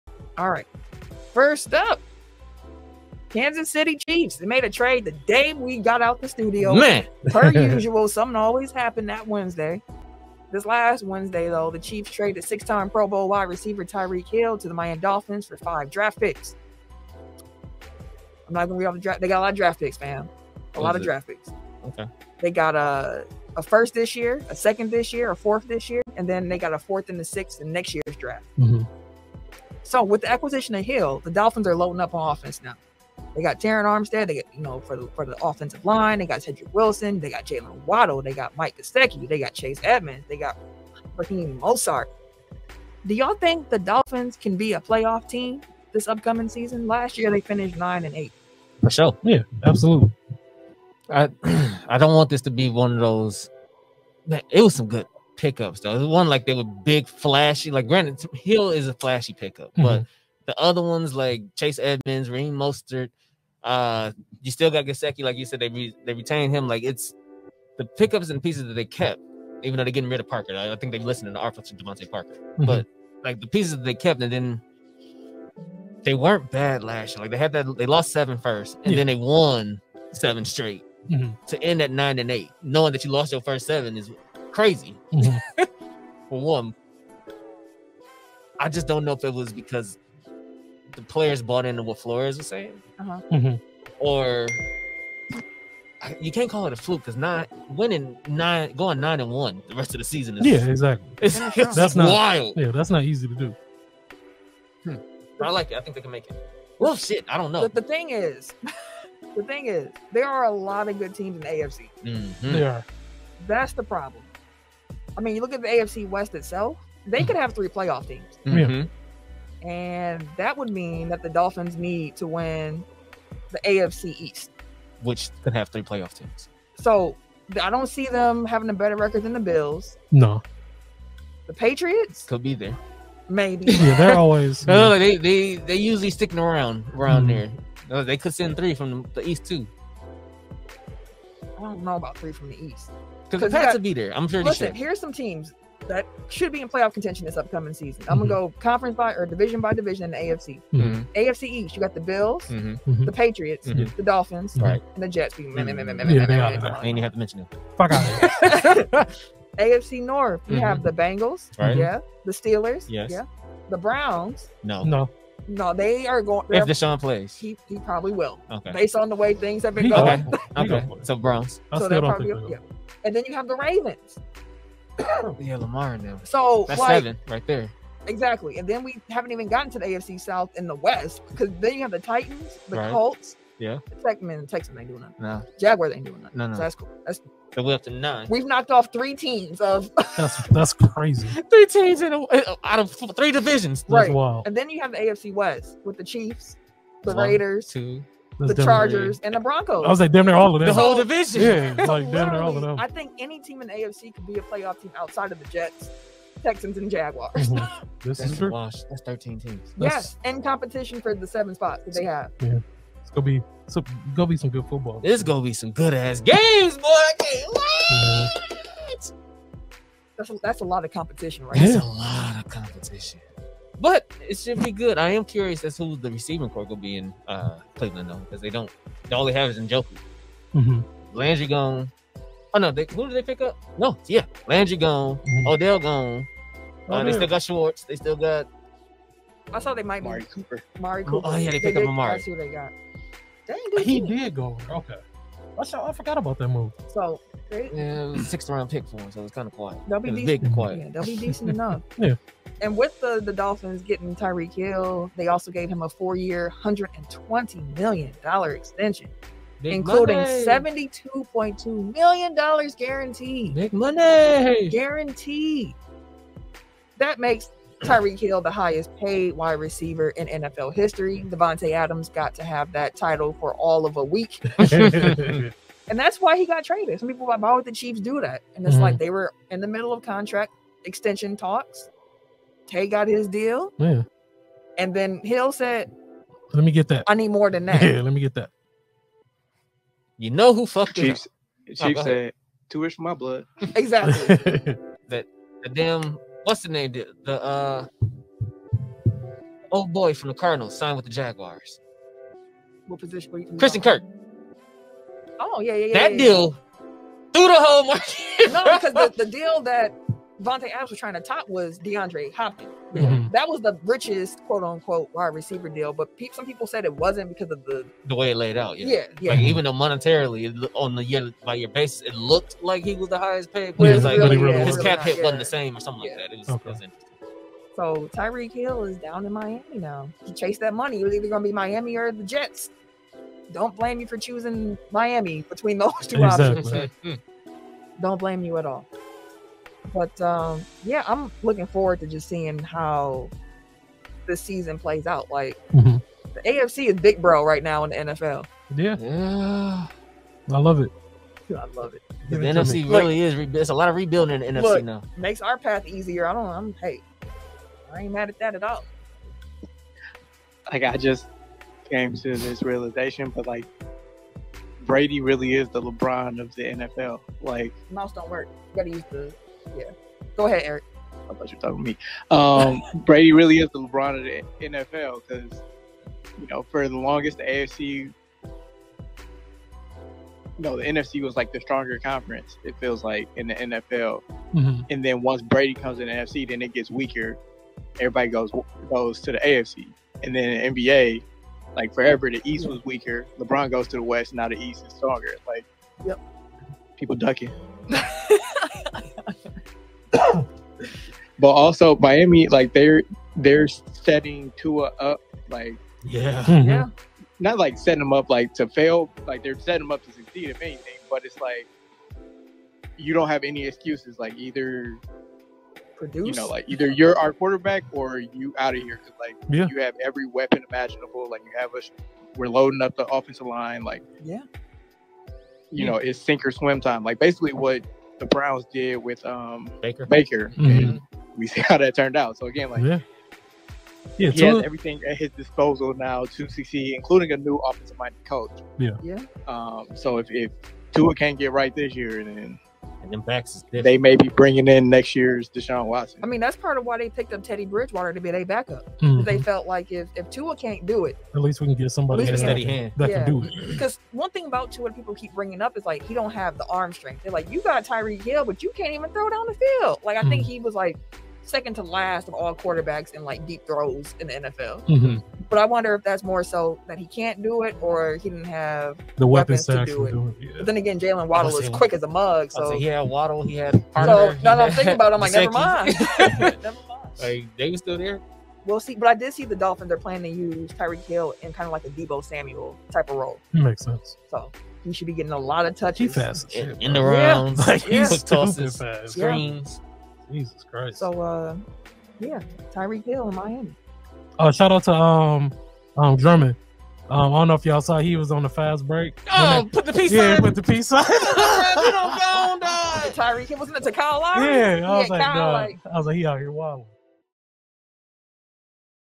The Dolphins traded for Tyreek Hill — are they playoff bound? | Episode 244 Clip
The Kansas City Chiefs traded six-time Pro Bowl wide receiver Tyreek Hill to the Miami Dolphins for five draft picks: a 2022 first-round pick (No. 29), second-round pick (No. 50) and fourth-round pick, plus fourth- and sixth-round picks in the 2023 draft on Wednesday. With the trade, the Dolphins also are giving Hill a four-year, $120 million extension, making Hill the highest-paid wide receiver in NFL history.

With the acquisition of Hill, the Dolphins are loading up on offense (OT Terron Armstead, WR Cedrick Wilson, WR Jaylen Waddle, TE Mike Gesicki, RB Chase Edmonds, RB Raheem Mostert), can the Dolphins can be a playoff team this season?